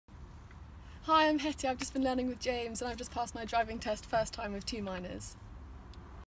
Driving Lessons Clayesmore School
Congratulations to Hetty Wraight from Clayesmore School near Shaftesbury Dorset on passing her driving test today 17/10/18, 1st time and with only 2 minor faults in Dorchester.

All the best and drive safe, from your driving instructor James Orgar and the rest of the team here at 2nd2None Driving School.

Hetty Wraight

Really enjoyed my lessons with James Orgar. Always felt calm and focused on the positives rather than the negatives and got me to pass first time so couldn’t of asked for anything more!!!